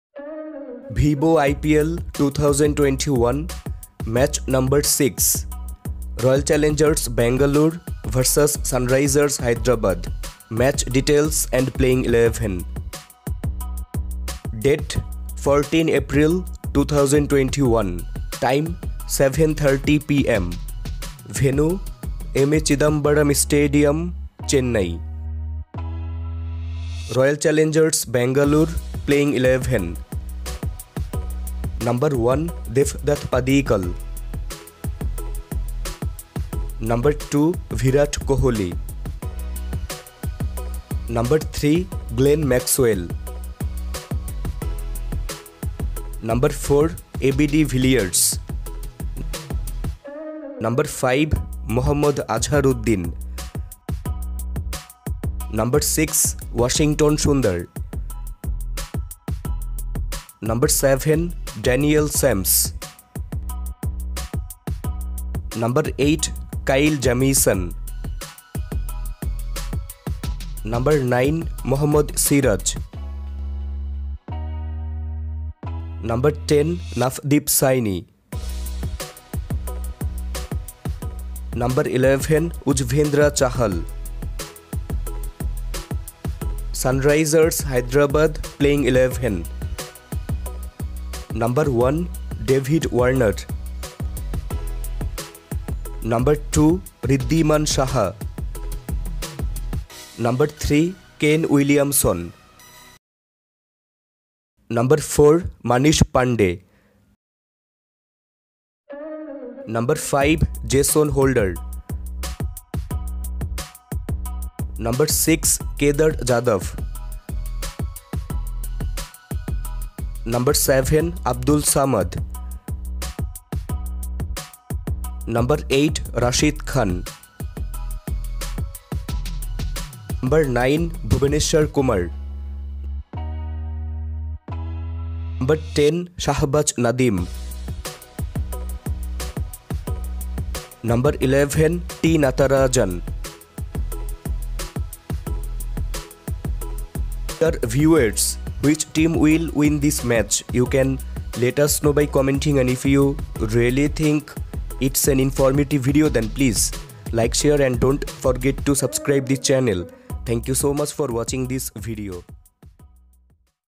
IPL 2021 Match 6 | Hyderabad Vs Bangalore 6th Match IPL 2021 Details & Playing 11 | SRH Vs RCB 2021
IPL 2021 Match 6 | Hyderabad Vs Bangalore 6th Match IPL 2021 Details & Playing 11 | RCB Vs SRH 2021

RCB Playing XI :

1. Devdutt Padikkal
3. AB de Villiers
4. Glenn Maxwell
5. Mohammed Azharuddeen (WK)
6. Daniel Christian
7. Washington Sundar
8. Kyle Jamieson,
9. Navdeep Saini
10. Mohammed Siraj &
11. Yuzvendra Chahal.

Sunrisers Hyderabad Playing XI..
1. David Warner
2. Wriddhiman Saha
3. Kane Williamson
4. Manish Pandey
5. Jason Holder
6. Kedar Jadhav
7. Abdul Samad
8. Rashid Khan
9. Bhuvneshwar Kumar
10. Shabaz Nadeem
11. T. Natarajan